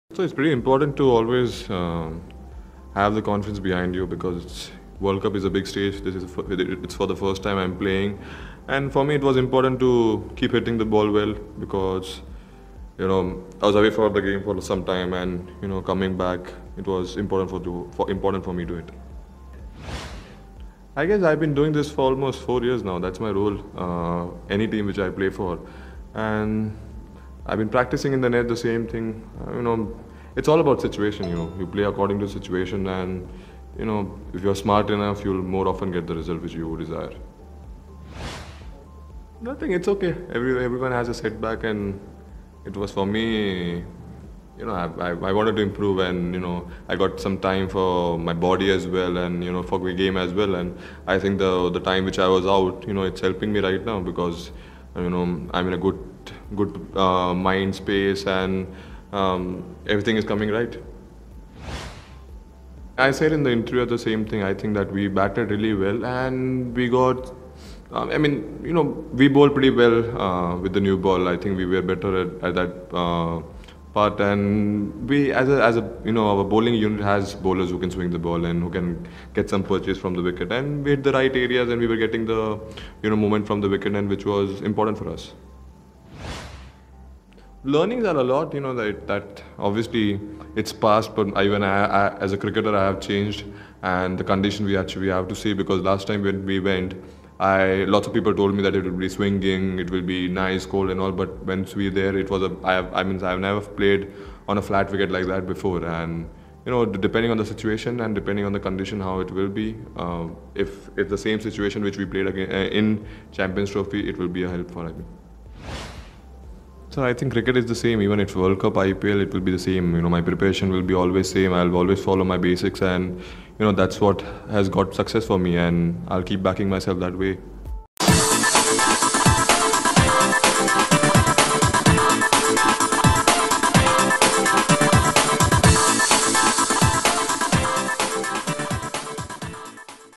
Time away from the game is helping me right now - Hardik Pandya
Mumbai all-rounder HardikPandya addresses the media after helping his side get over the line against Bangalore